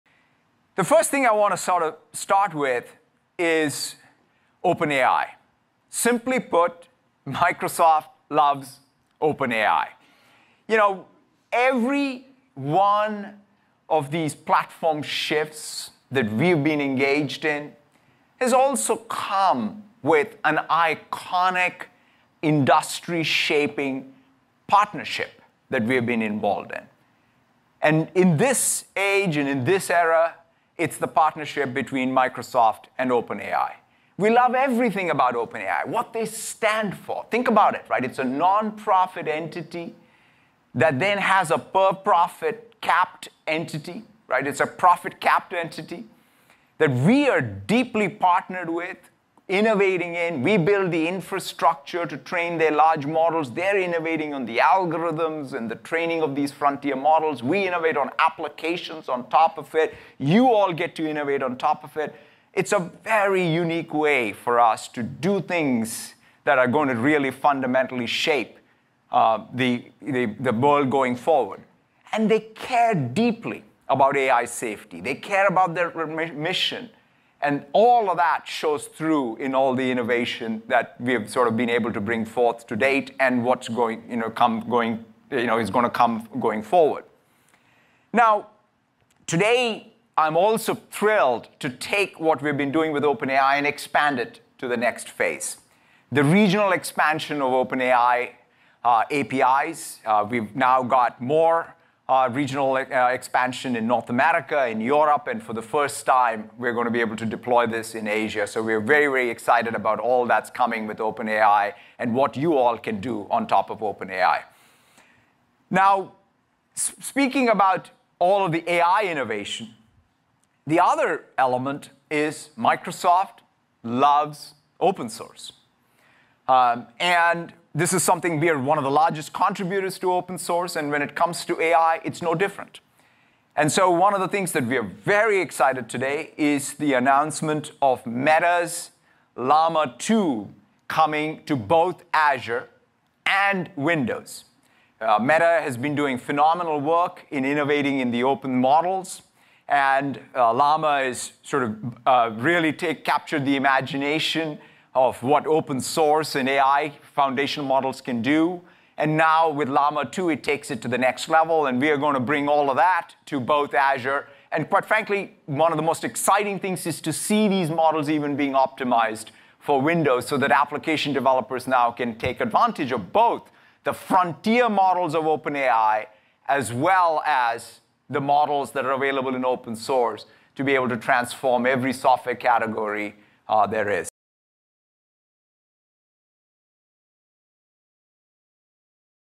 Microsoft and Meta Team Up on Open-Source AI Tool (Llama 2)
Watch Microsoft CEO Satya Nadella announce the company’s partnership with Meta to incorporate the open source Llama 2 LLM into Windows.